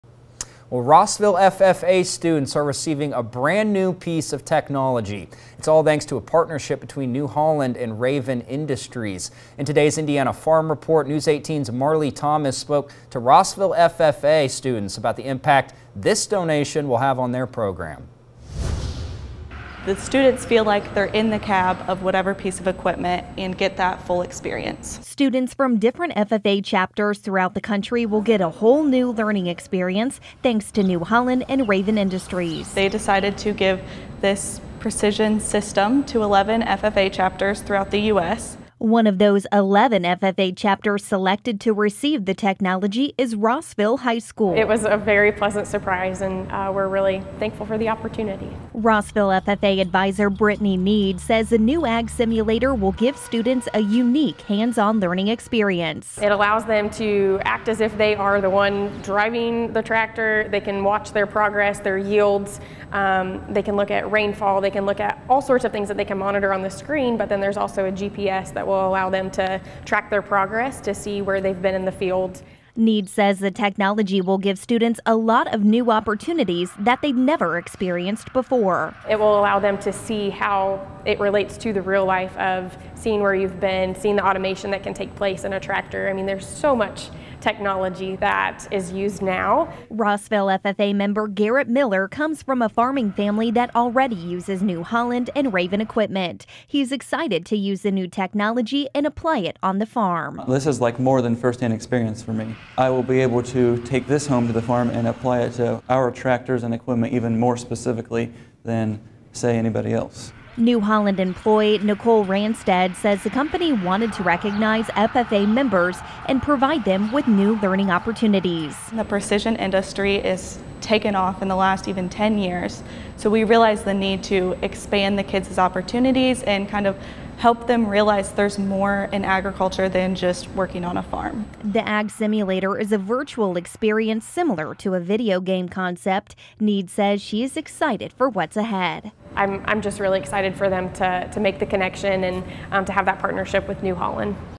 New Holland and Raven Industries donate new technology to Rossville FFA
Rossville FFA members will soon get a new hands-on learning experience, thanks to New Holland and Raven Industries. The Rossville FFA program is the only chapter in Indiana set to receive the technology.